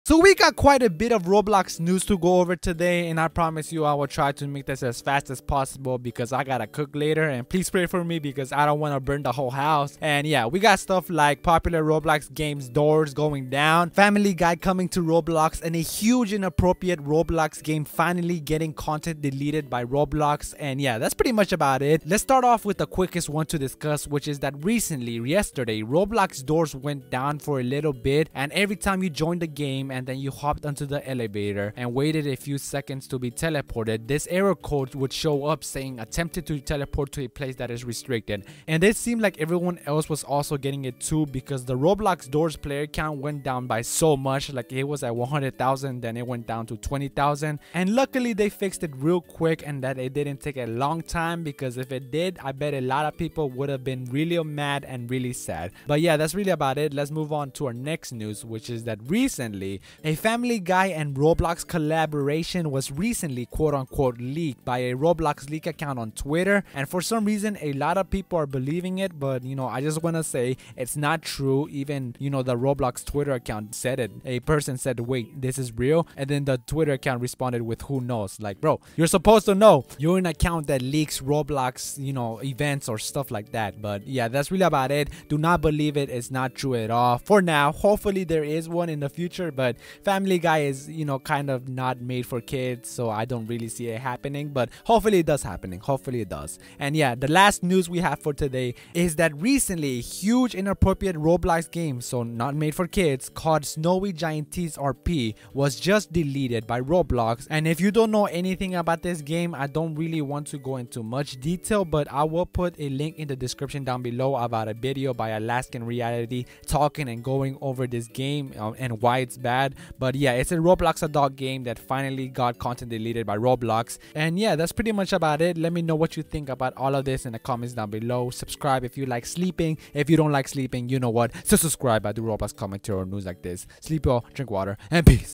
HUGE Roblox Adult game finally got banned...
In todays roblox news video we talk about, roblox 18+ game banned, roblox and family guy, roblox going down, and it gets really crazy with some news actually being kind of good but showing how roblox moderation is never taken seriously